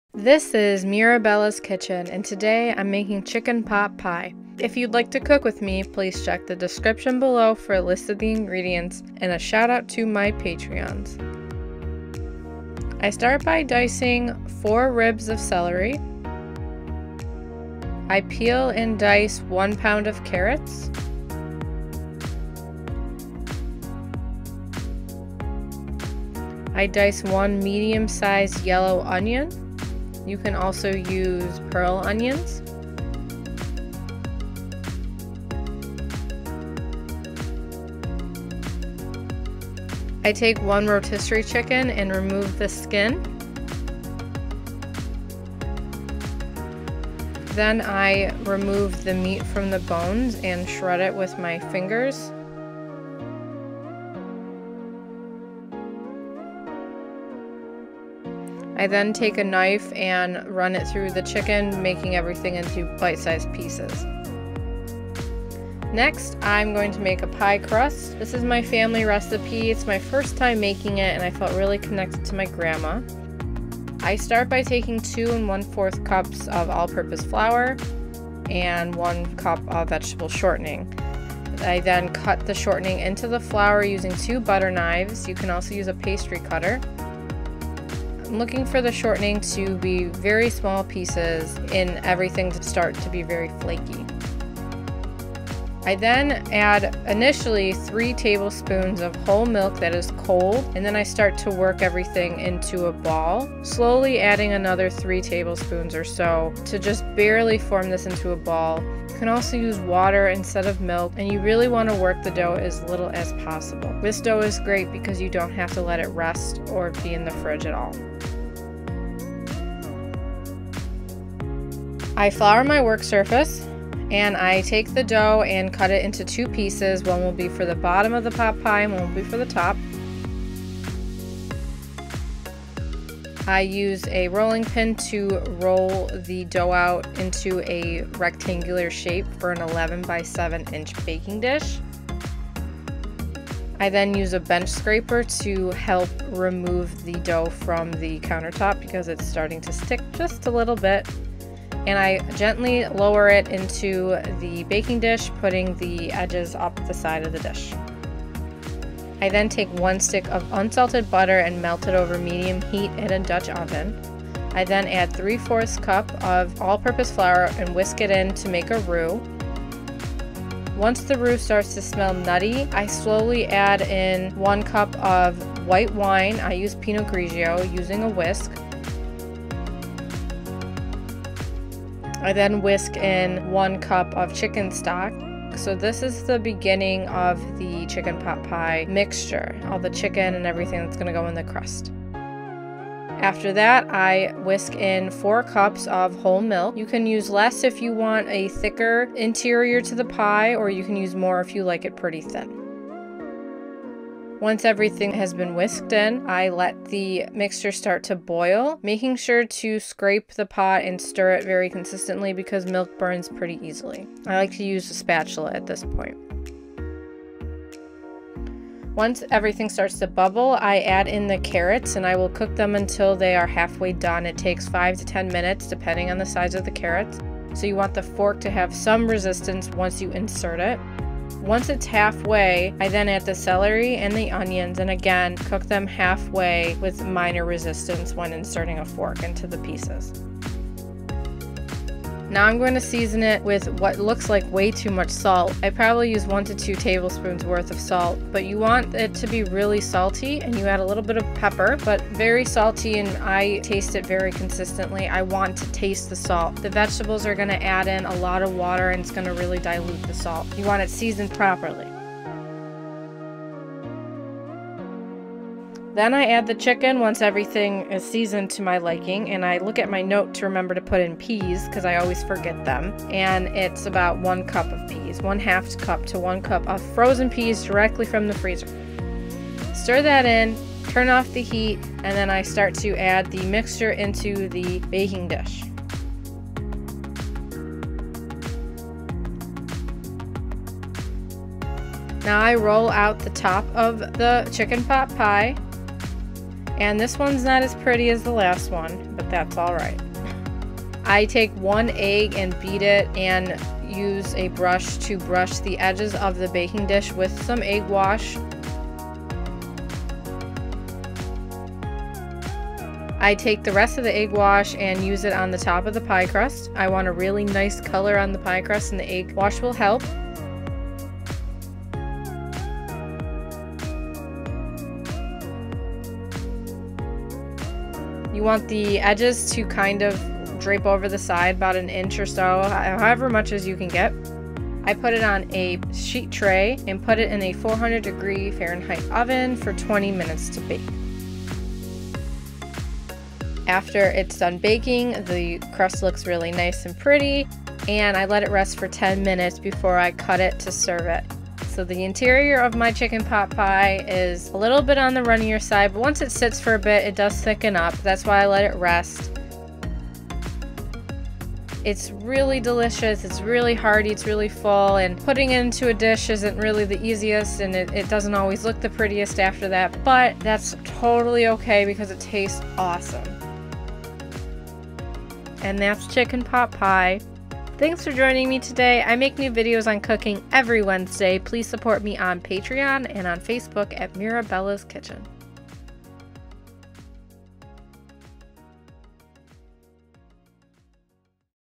Mirabella's Kitchen: Chicken Pot Pie
I love chicken pot pie. I have long graduated from the days of microwaving Marie Callender. Pot pie is a small undertaking if making the pie crust from scratch, but definitely worth it. I enjoy my pot pie with a creamy filling compared to a stock filling. To spice your pie crust up - add a sprinkling of oregano. Your vegetable filling can be any combination of vegetables you'd like as well!

Servings: 6-8

Prep time:  30 minutes
Cook time: 60 minutes
Total time: 90 minutes

Ingredients:
Filling -
4 ribs celery, diced
1 lb carrots, peeled and diced
1 medium yellow onion, diced
1 rotisserie chicken, skin removed and meat cut into bite sized pieces
1 stick unsalted butter, melted
3/4 cup all-purpose flour
1 cup white wine
1 cup chicken stock
4 cups whole milk
1-2 Tbsp coarse kosher salt
1/2 tsp black pepper
1/2 - 1 cup frozen peas, still frozen
Pie crust -
2 & 1/4 cups all-purpose flour
1 cup vegetable shortening
5-6 Tbsp cold whole milk (or water)
1 egg, beaten for egg wash on pie top/brushing the edges of the baking dish